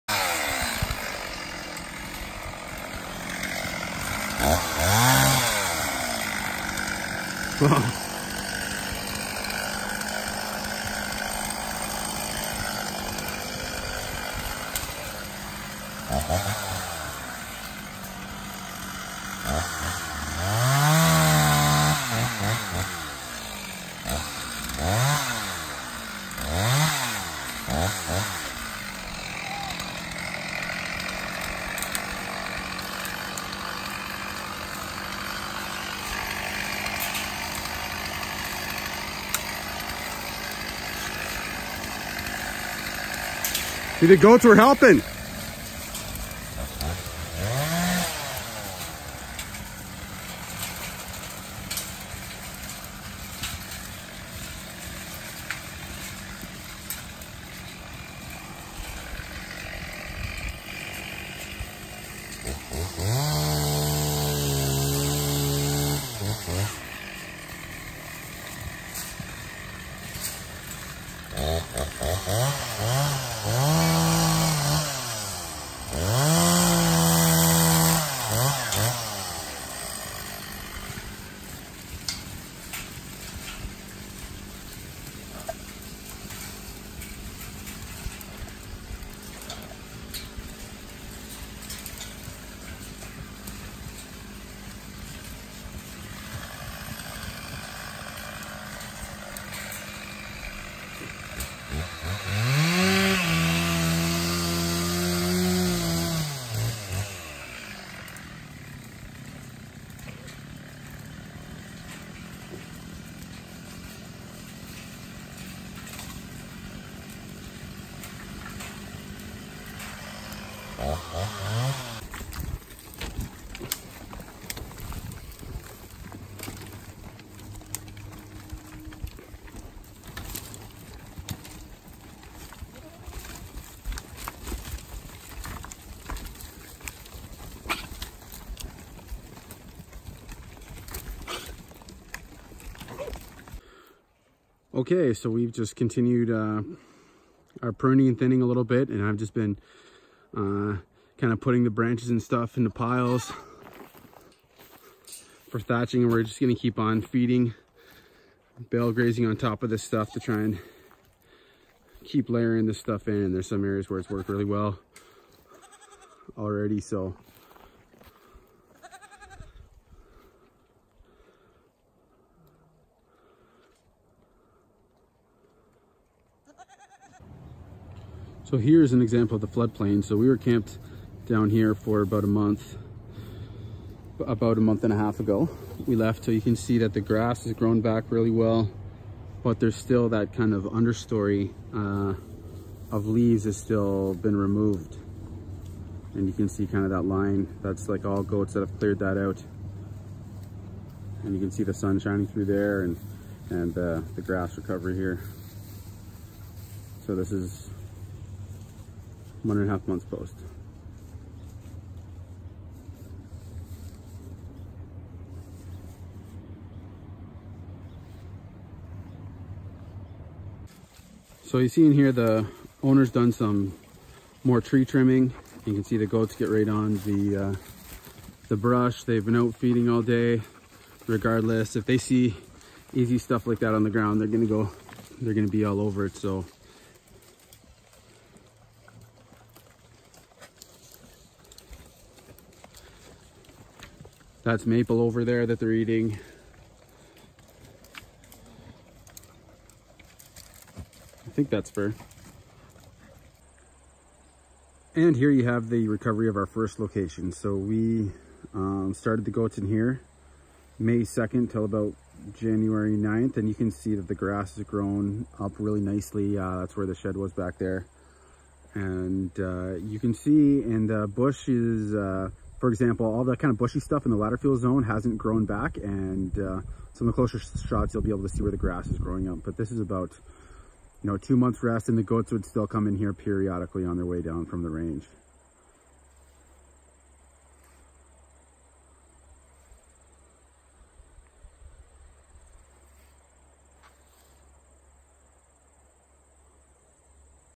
Goats in Fuel Mitigation and Site Recovery
Goats feed on the slash and vegetative debris, redeucing the need for burning.  This enhances soil bioliogy and site recovery.